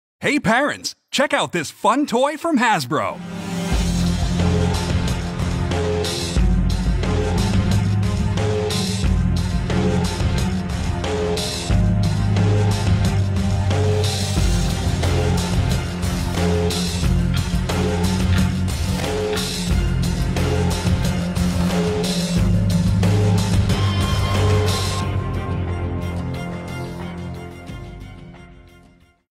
Nerf Modulus Ghost Ops Evader (Ghost Gun)
Nerf Modulus Ghost Ops Evader

Stay in stealth mode or light the way
Clear blaster for stealth missions
Blaster illuminates for low-light battles
Motorized blaster comes with 12 darts
Includes light-up barrel extension